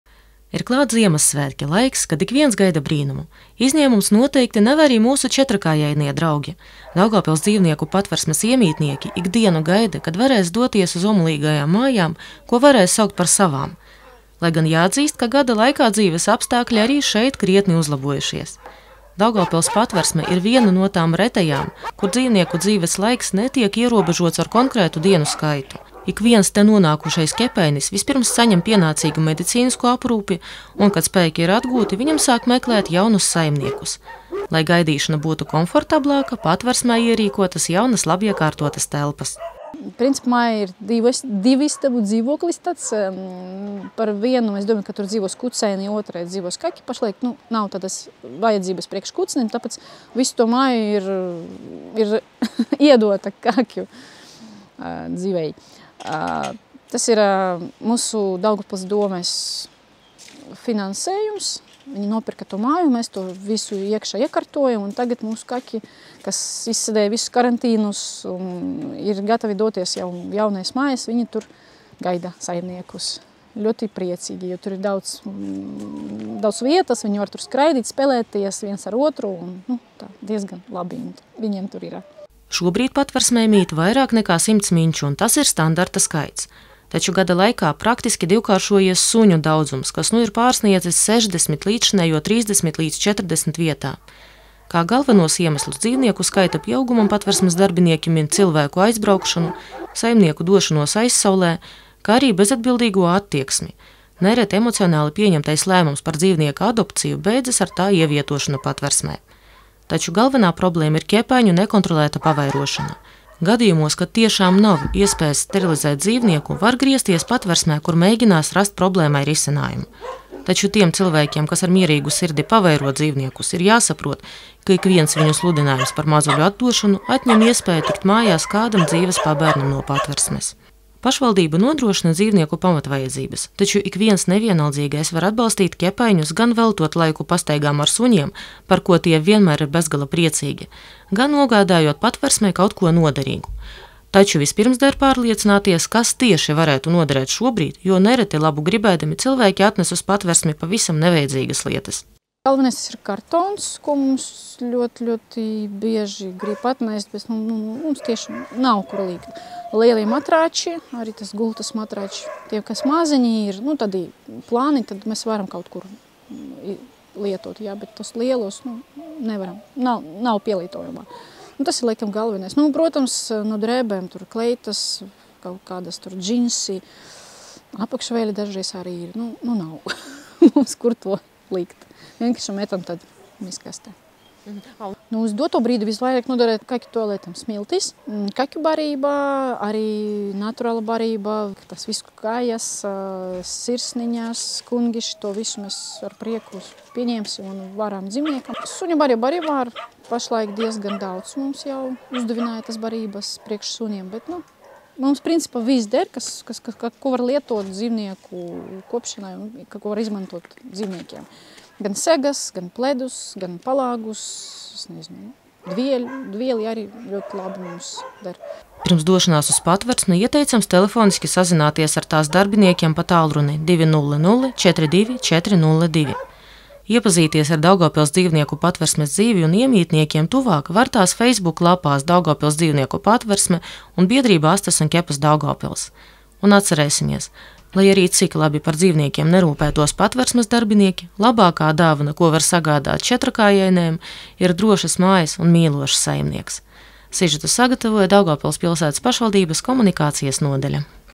Dzīvnieku patversmes iemītnieki gaida tikšanos ar saimniekiem
Ir klāt Ziemassvētki – laiks, kad ikviens gaida brīnumu. Izņēmums noteikti nav arī mūsu četrkājainie draugi: Daugavpils dzīvnieku patversmes iemītnieki ik dienu gaida, kad varēs doties uz omulīgajām mājām, ko varēs saukt par  savām. Lai gan jāatzīst, ka gada laikā dzīves apstākļi arī šeit krietni uzlabojušies.